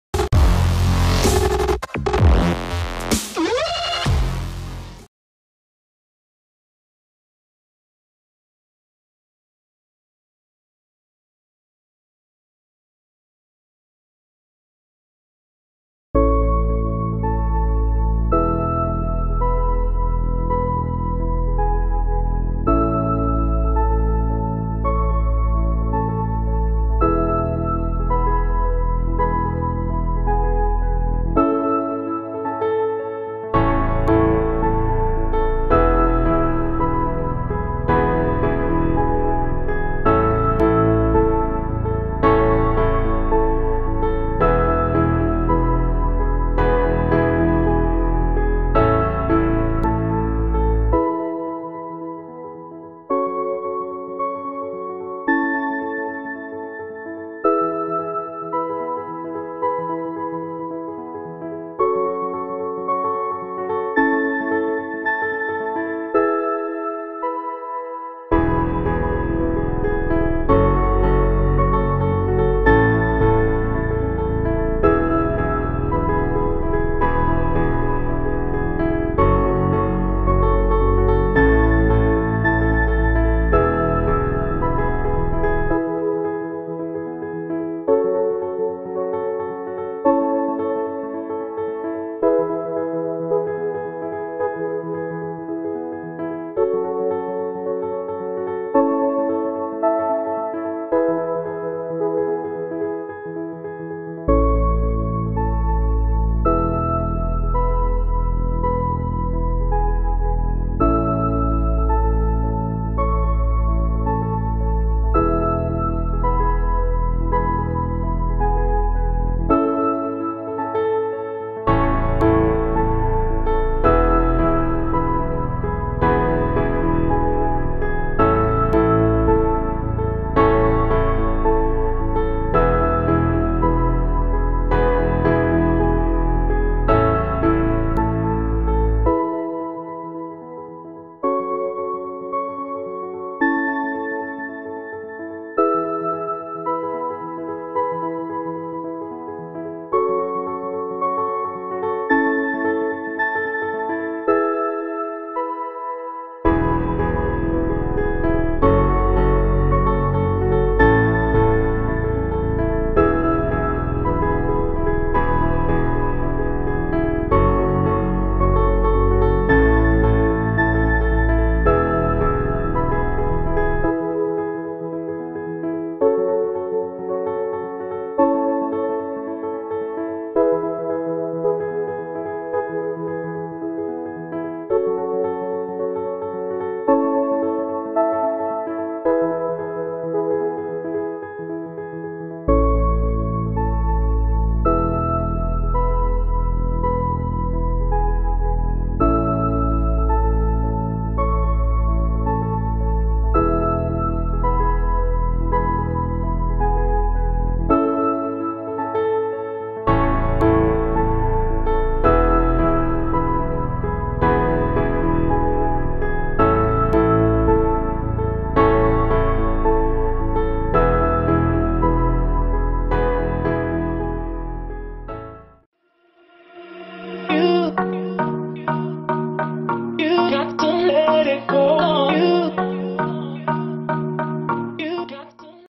PRAYER FOR PROTECTION | PSALM 91
"Psalm 91, Verses 10 and 11"

"No harm will come to you,
no misfortune will reach your home.
Because He will give orders to your angels about you, that they may protect you in all your ways."